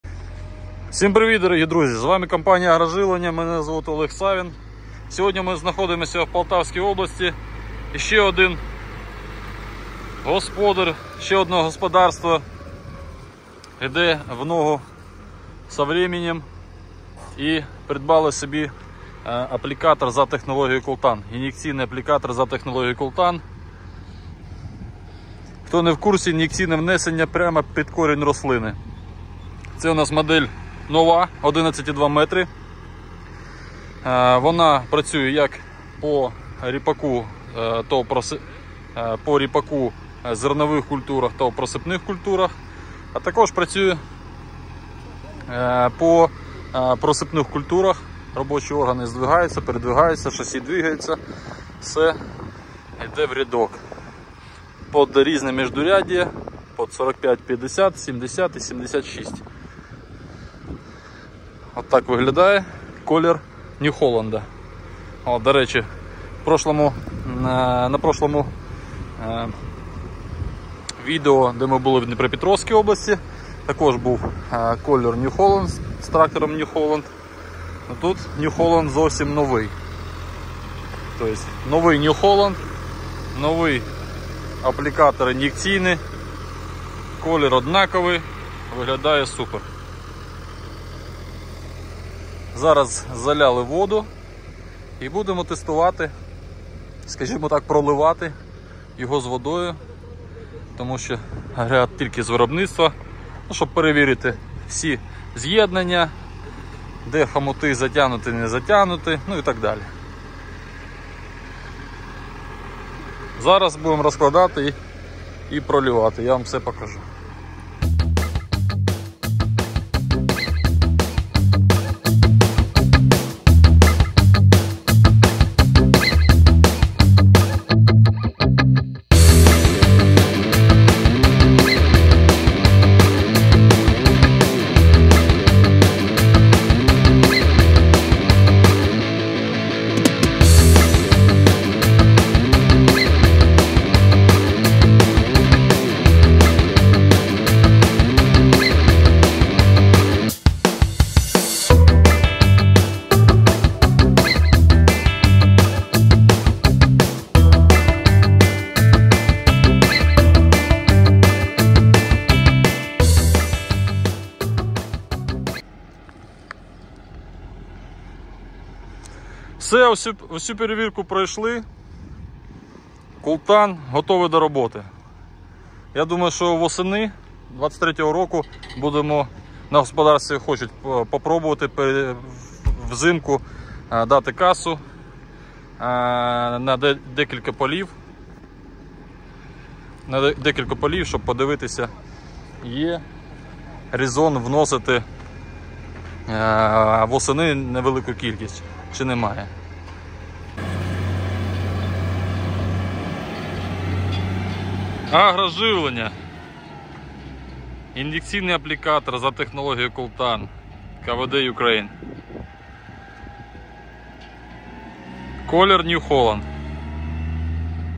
Аплікатор АПІ-11,2 метра Полтавська обл.
Друзі усім привіт!!! Нова модель Причіпний Аплікатор для внесення рідких добрив АПІ-11,2 Cultan набирає обертів!!! Хто планує працювати за сучасною технологією внесення рідких добрив "CULTAN" замовляйте у нас вже сьогодні.
• 11,2 метра ширина захвату
• Ємність 3000 літрів
• 44 робочих органу KWD Germany
• Електроніка ARAG (Італія)
• Норма внесення від 100-2000 л/га
• Шасі дає можливість працювати по
просапних культурах.
• Швидкість внесення 8-10 км/год.
• Продуктивність 11 га/год
Наші контакти:
ТОВ "Агро Живлення"